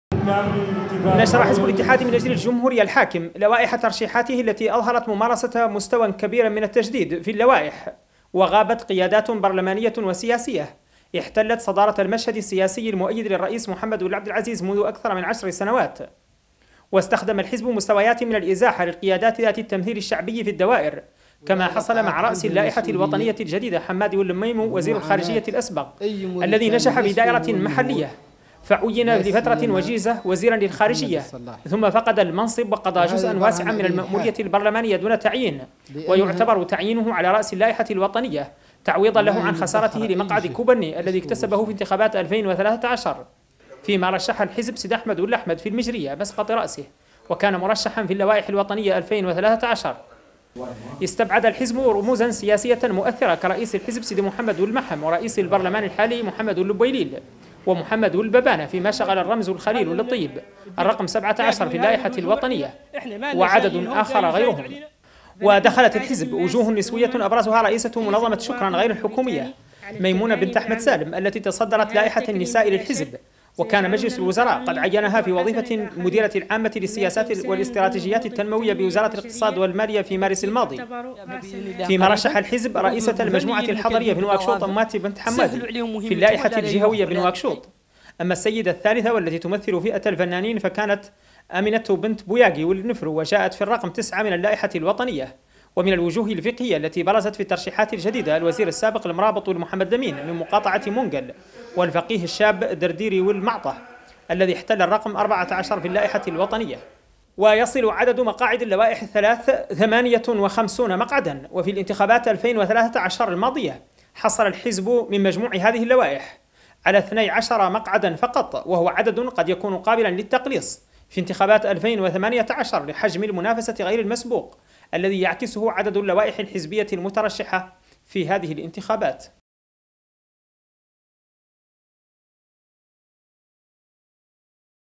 الاتحاد من أجل الجمهورية يعلن عن قوائم ترشيحاته للانتخابات التشريعية تقرير الحافظ ولد الغابد.
أعلن حزب الاتحاد من أجل الجمهورية الحاكم في موريتانيا عن قوائم مرشحيه للائحتين الوطنيتين، واللائحة الجهوية لنواكشوط، استعدادا للانتخابات المرتقب تنظيمها فاتح سبتمبر القادم.
وقد شهدت لوائح الترشحات، والتي نشرها الحزب على موقعه الألكتروني، استبعادا لعدد من البرلمانيين، الذين جدد لهم خلال المأمورية الماضية، كما عرفت ترشيح اثنين من أعضاء مجلس الشيوخ المنحل.